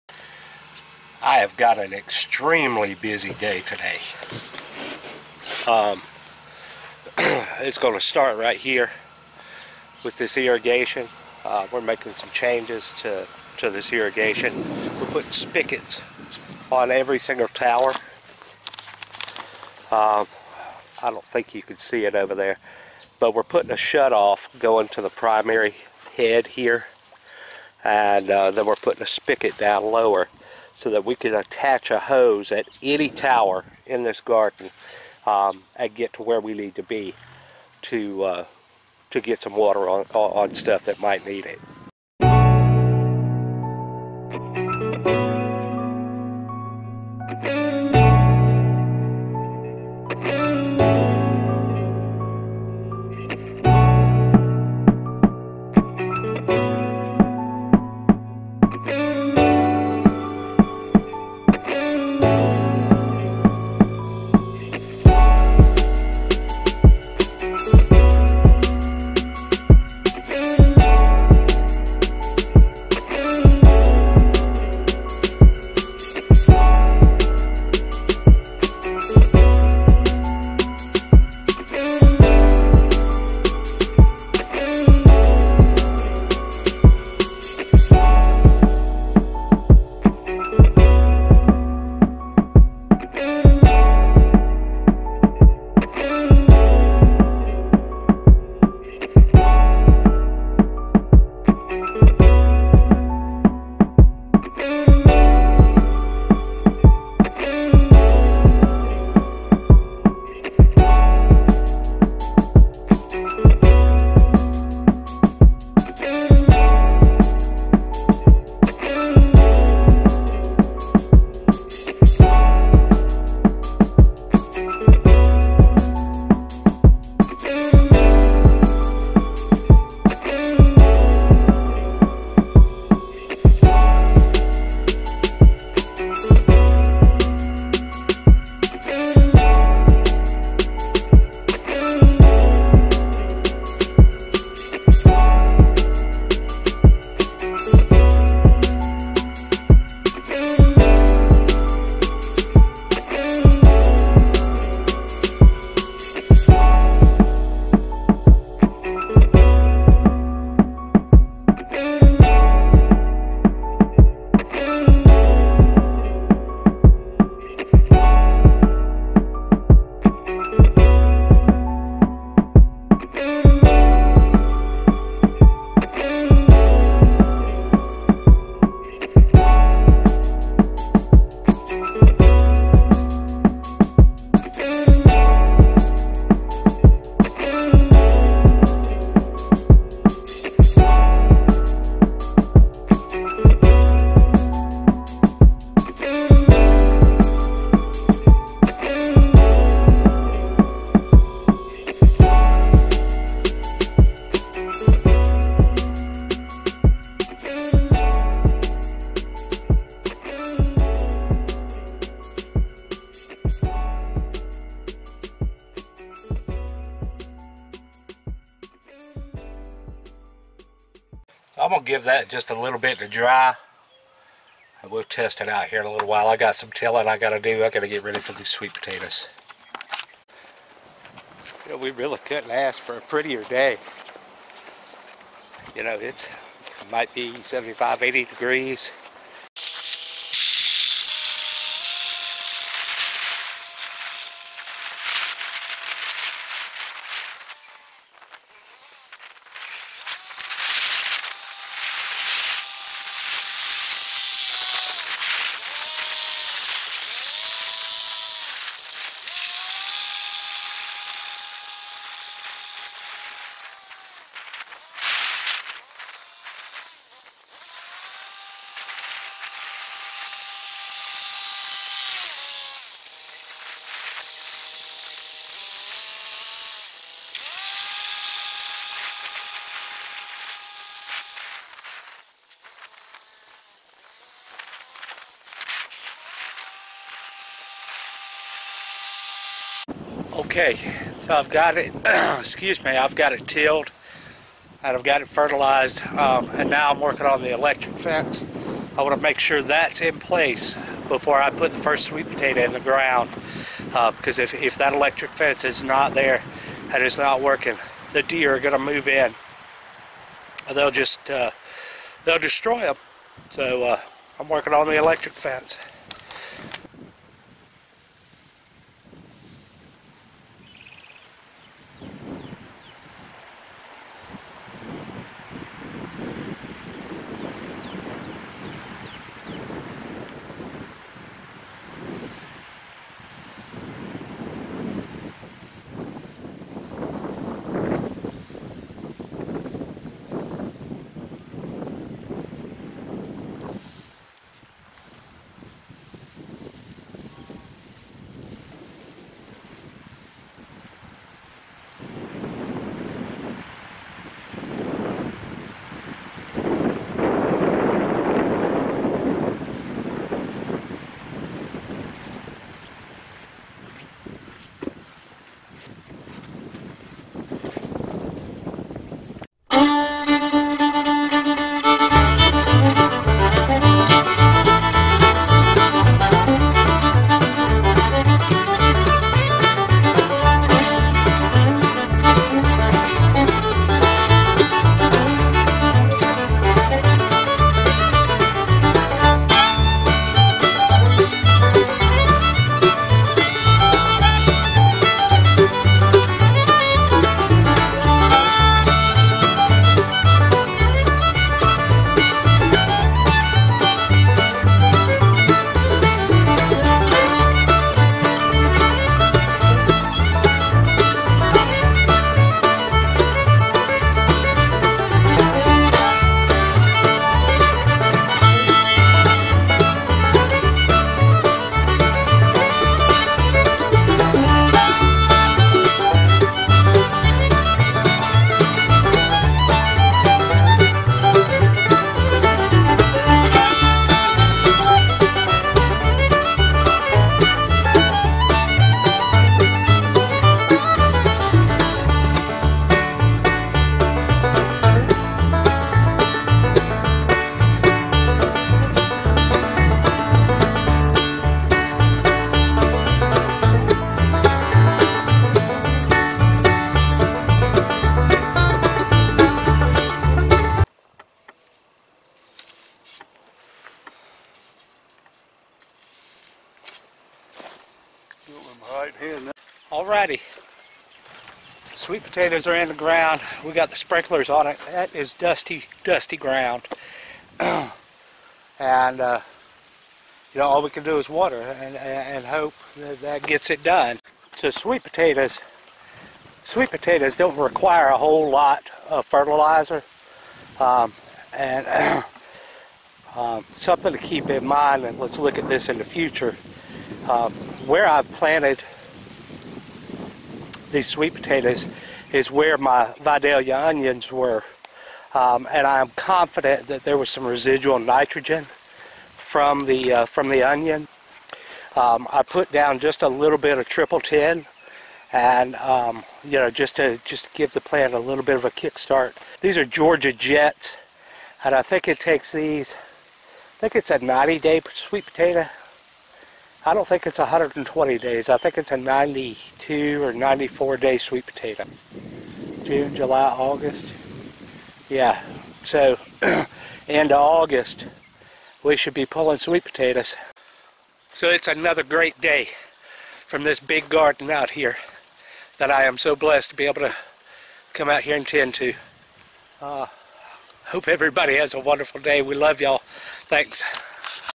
Irrigation modifications and planting sweet potatoes
We made some major upgrades to the irrigation system at the big garden.  We also got our Georgia Jet sweet potatoes planted.